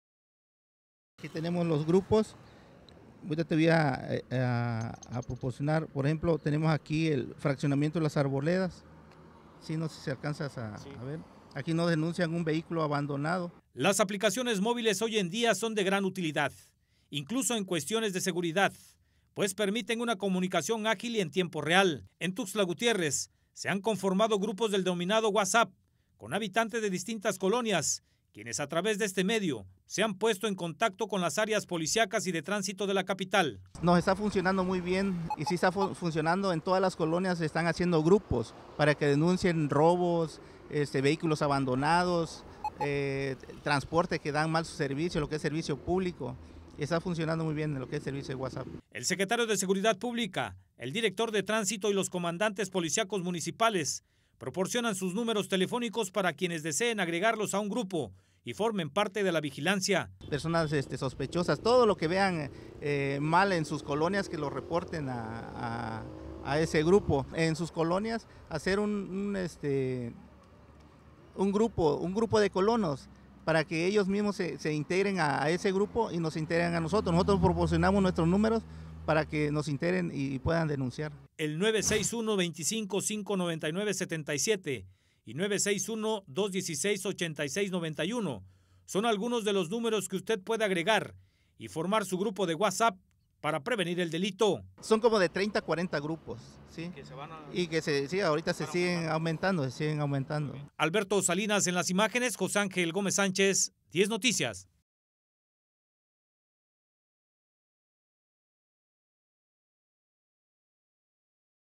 Crean grupos de whatsapp para reportar anomalías a policía municipal
Las aplicaciones móviles hoy en día son de gran utilidad, incluso, en cuestiones de seguridad, pues permiten una comunicación ágil y en tiempo real.
En Tuxtla Gutiérrez se han conformado grupos del denominado whatsapp con habitantes de distintas colonias, quienes a través de este medio, se han puesto en contacto con las áreas policíacas y de tránsito de la capital.